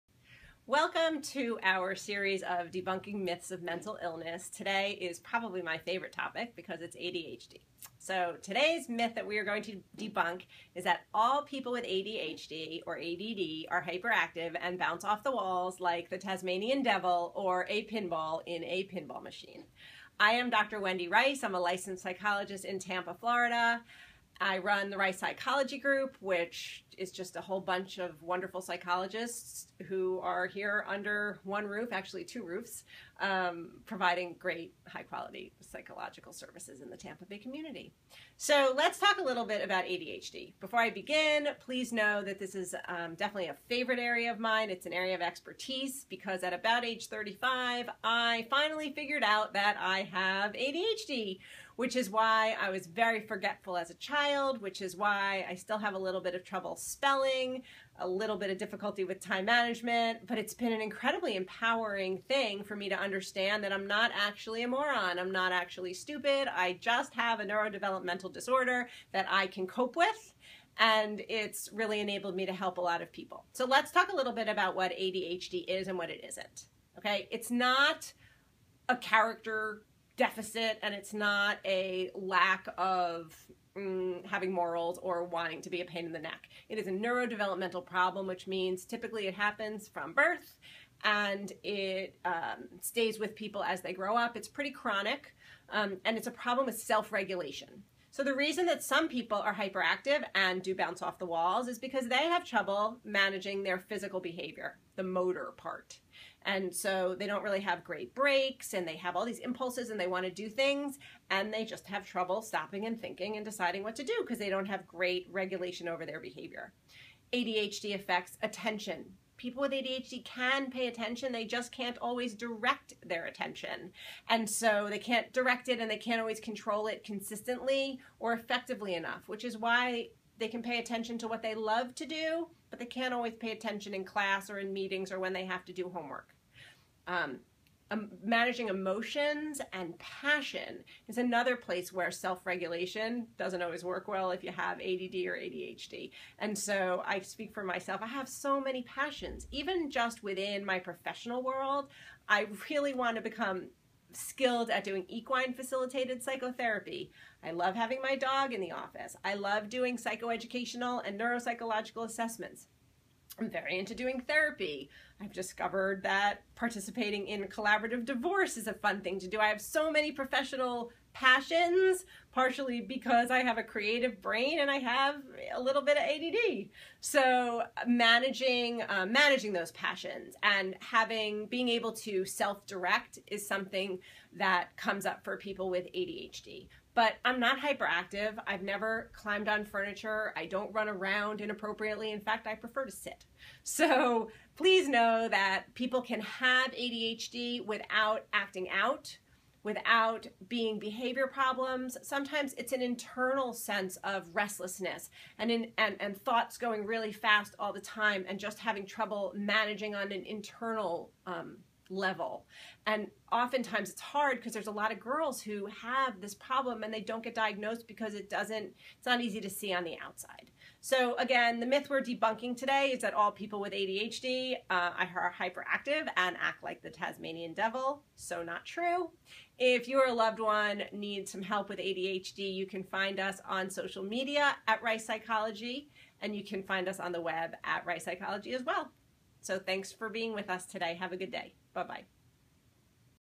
Debunking Myths of Mental Illness Series: ADHD
There are countless  myths and misunderstandings circulating around the topics of ADD and ADHD.  In this segment, Dr. Wendy Rice explains the realities of ADD and ADHD as well as how it affects her daily life.